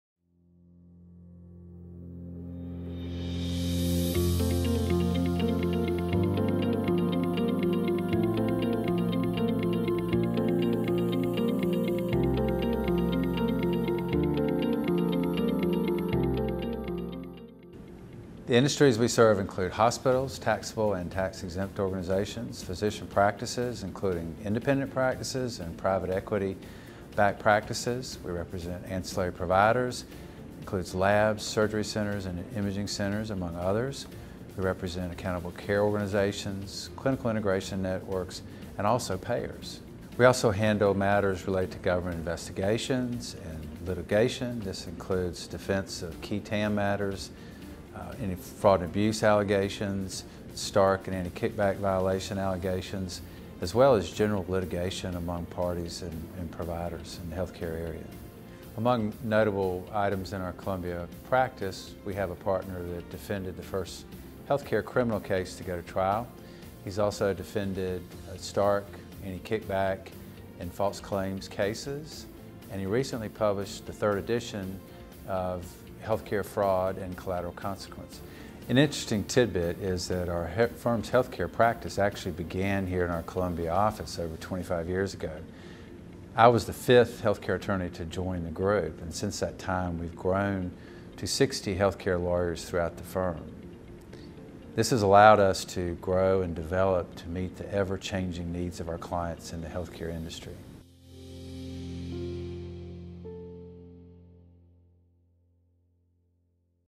Columbia Office Spotlight: Evolving for the Needs of a Nuanced Industry
The Nelson Mullins Healthcare Team was formed in Columbia, but over time it has rooted itself in many offices around the country. Columbia Partner Ed White was the fifth healthcare attorney to join Nelson Mullins, and he is proud to see the team grow in response to the needs of the industry, with over 60 healthcare lawyers throughout the firm today.

Transcript:
The industries we serve include hospitals, taxable and tax-exempt organizations, physician practices, including independent practices and private equity backed practices. We represent ancillary providers — that includes labs, surgery centers, and imaging centers, among others. We represent accountable care organizations, clinical integration networks, and also payers. We also handle matters related to government investigations and litigation. This includes defense of qui tam matters, any fraud and abuse allegations, Stark and Anti-Kickback violation allegations, as well as general litigation among parties and providers in the healthcare area. Among notable items in our Columbia practice, we have a partner that defended the first healthcare criminal case to go to trial. He’s also defended Stark, Anti-Kickback, and false claims cases. And he recently published the third edition of Healthcare Fraud and Collateral Consequences. An interesting tidbit is that our firm’s healthcare practice actually began here in our Columbia office over 25 years ago. I was the fifth healthcare attorney to join the group and since that time we’ve grown to 60 healthcare lawyers throughout the firm. This has allowed us to grow and develop to meet the ever-changing needs of our clients in the healthcare industry.